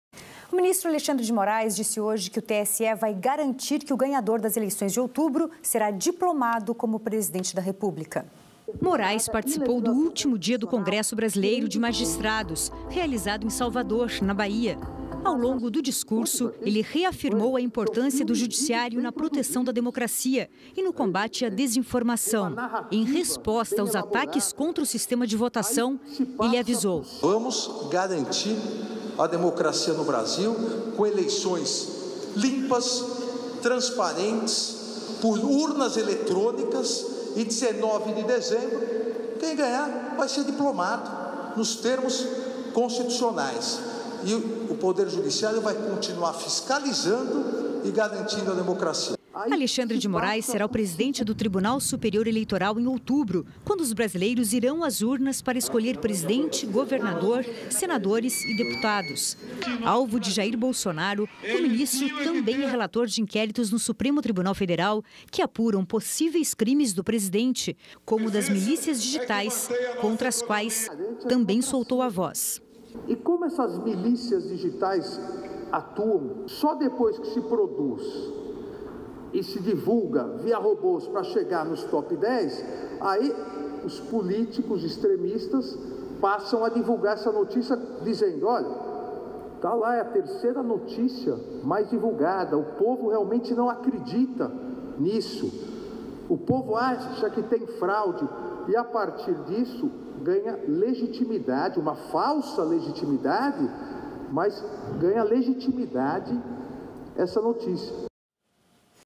Alexandre de Moraes: "Vamos garantir a democracia no Brasil com eleições limpas"
O ministro do Supremo Tribunal Federal Alexandre de Moraes, que será o presidente do TSE durante as eleições de 2022, reafirmou nesse sábado (14) que o Poder Judiciário “vai continuar fiscalizando e garantindo a democracia no Brasil”.

"Nós vamos garantir a democracia no Brasil com eleições limpas, transparentes, por urnas eletrônicas e, em 19 de dezembro, quem ganhar vai ser diplomado nos termos constitucionais", diz ministro do STF Alexandre de Moraes